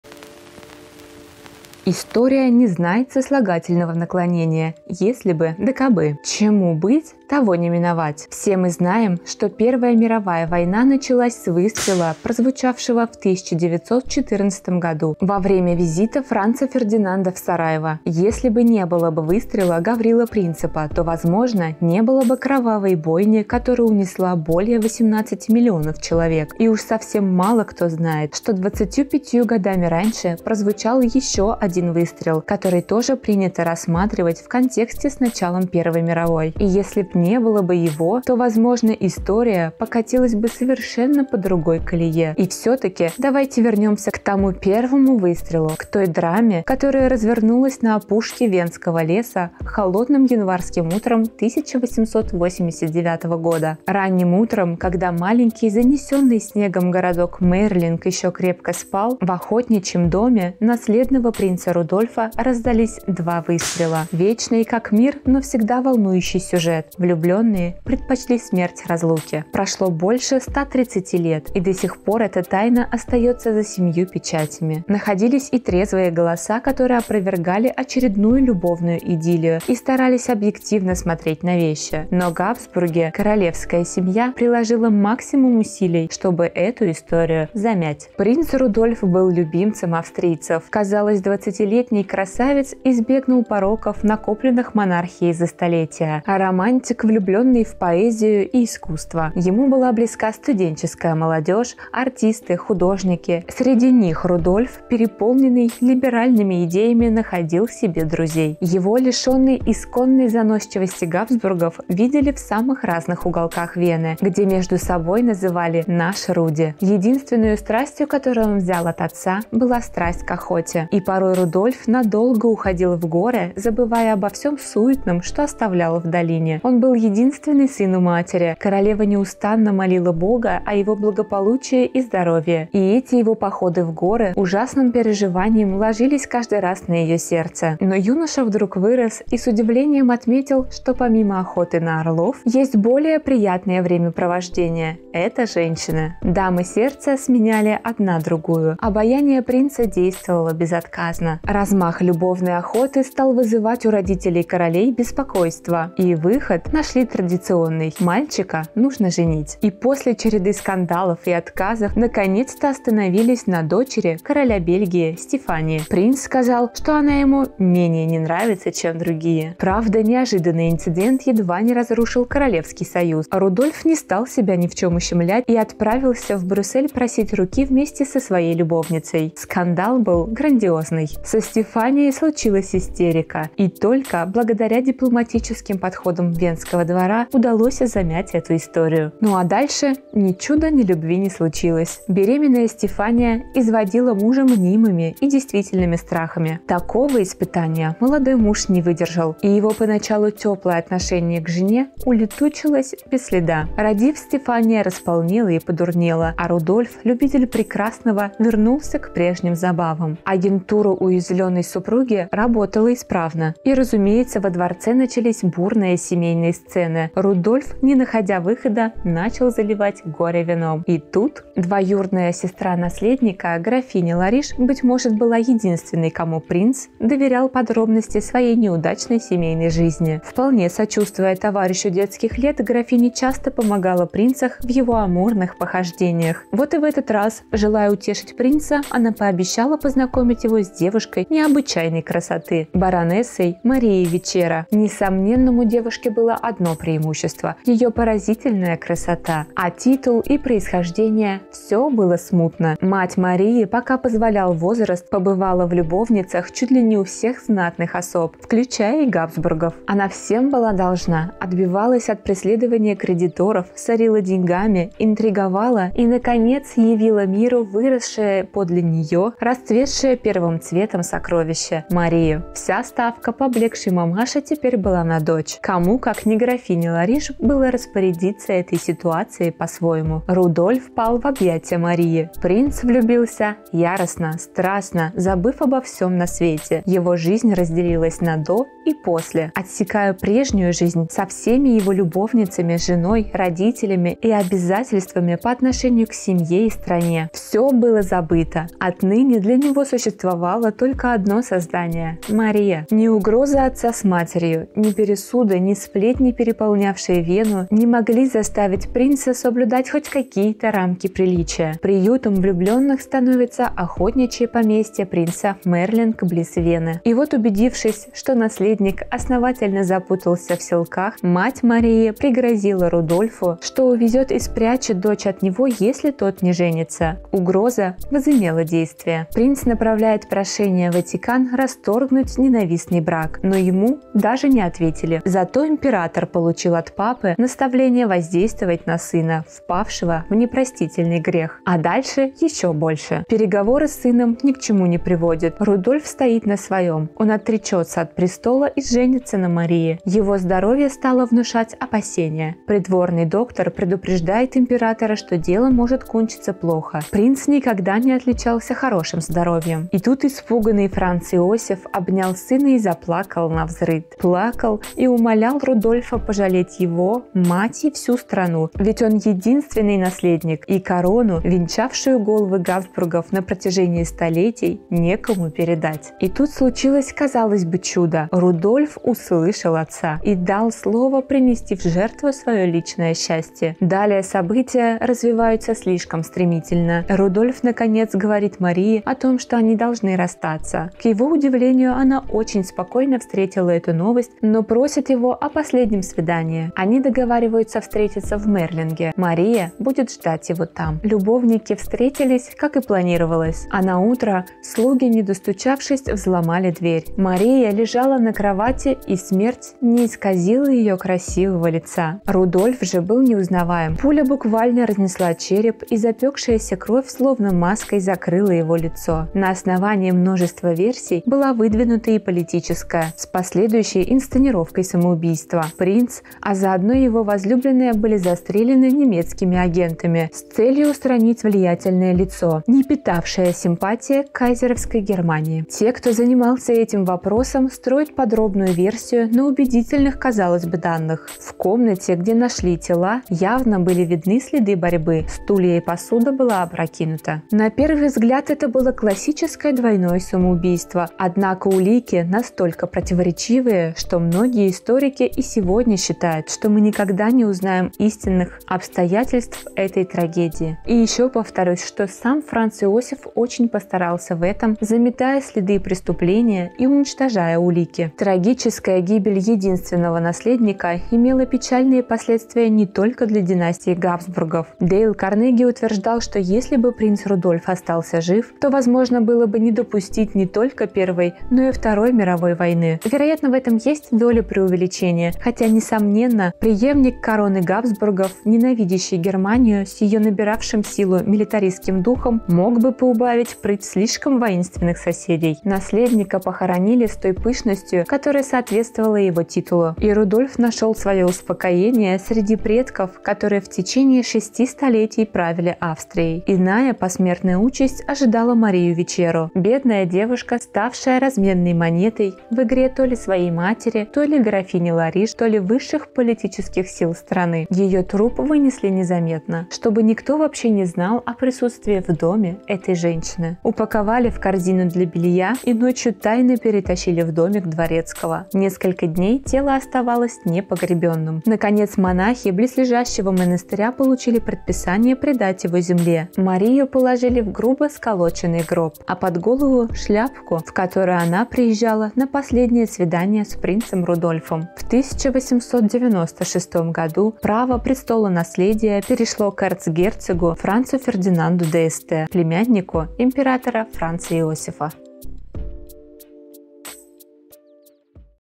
Роковая любовь принца Рудольфа и Марии Вечера
Тайна выстрелов в замке Майерлинг до сих пор порождает много слухов и домыслов. Кто такая Мария Вечера? Случайно ли она появилась в жизни кронпринца? И какую все-таки роль сыграла в его судьбе.